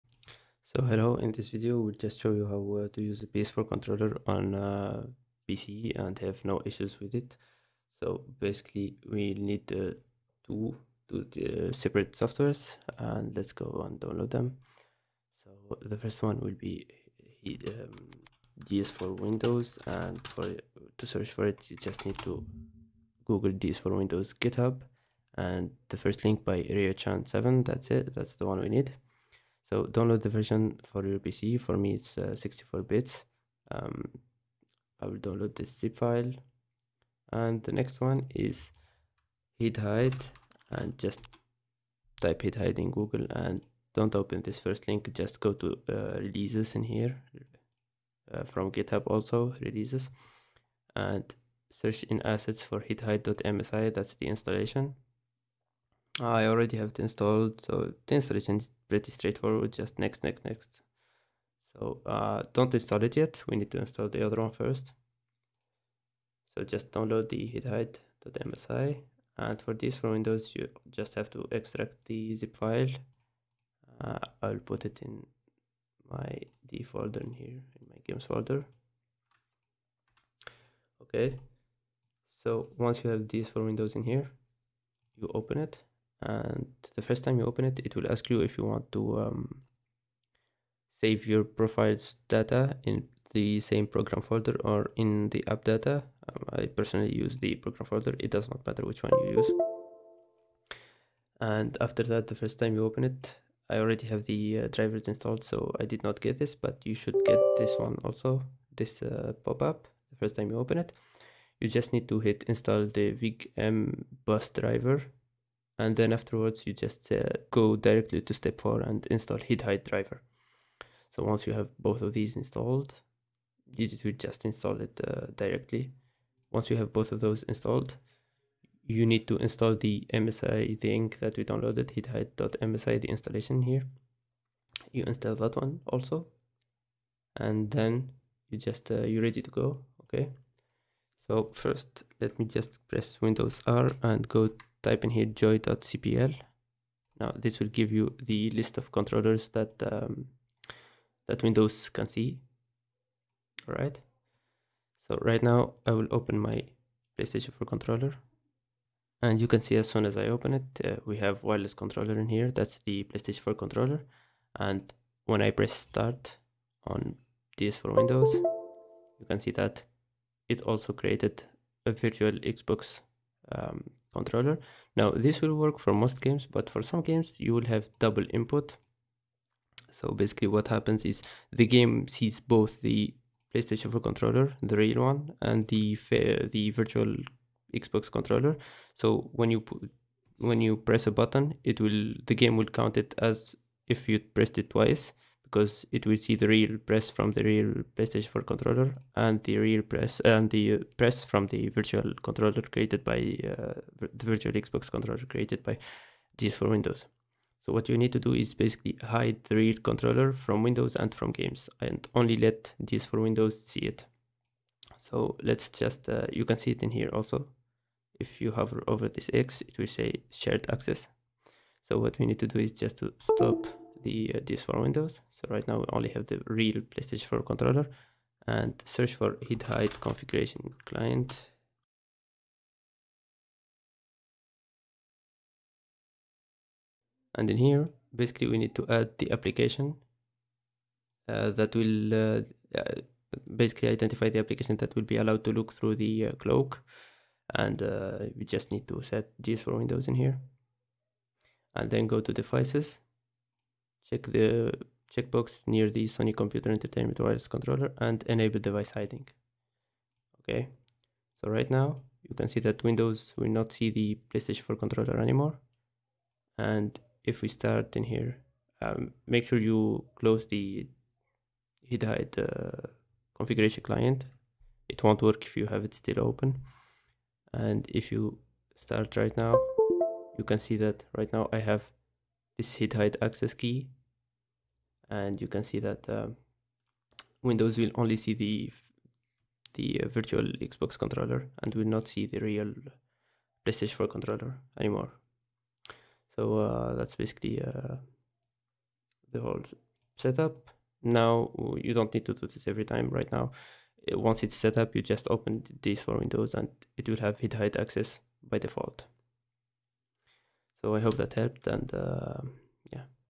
How to play with ps4 controller on pc - DS4windows & Hidhide [ no double input ]
This also solves the infinite turning issue in sekiro and other games .